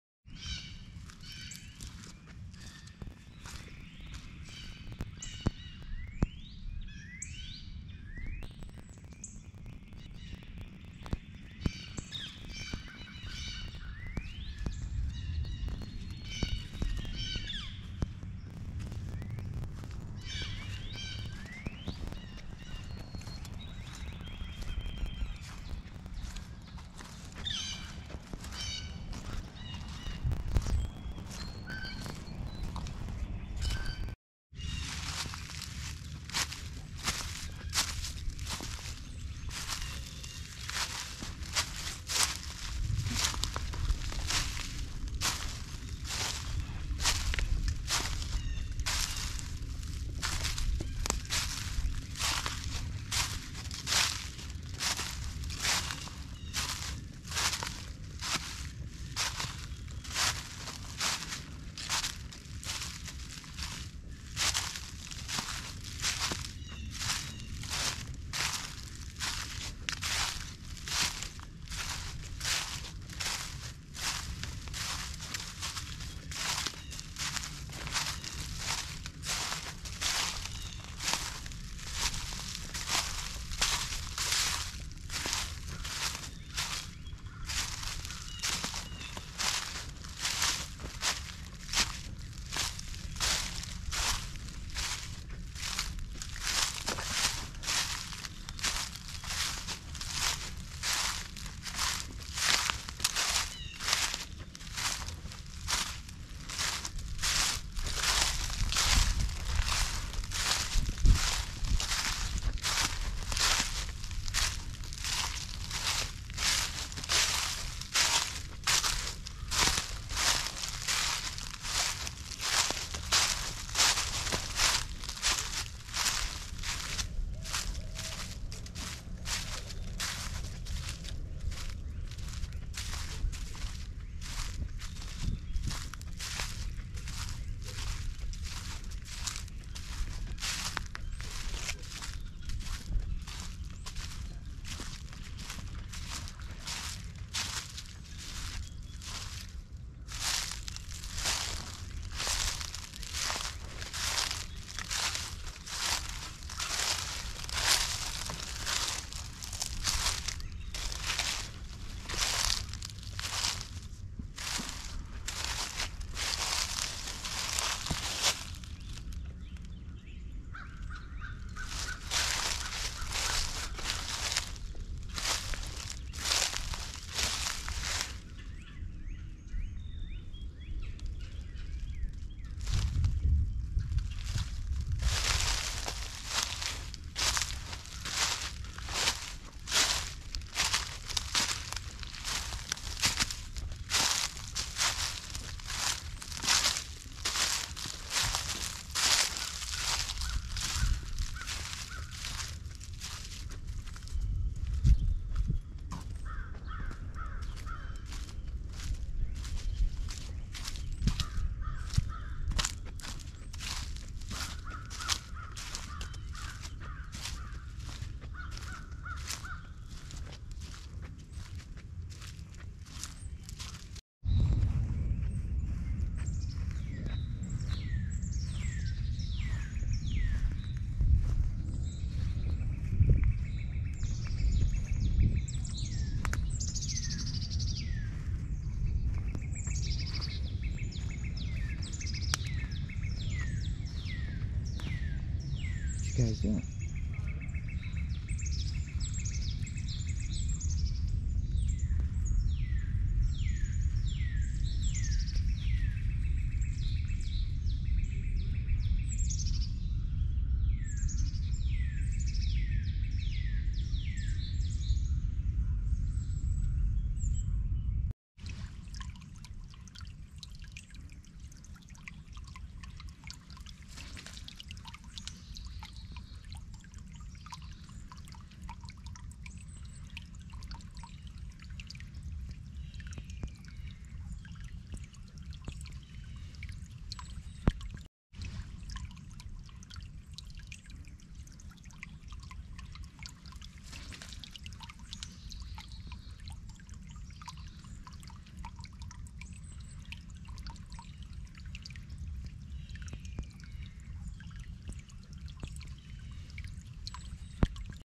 Adventuring outside and the Deer come to visit.
Walking in the woods and yard. Can you see the Deer?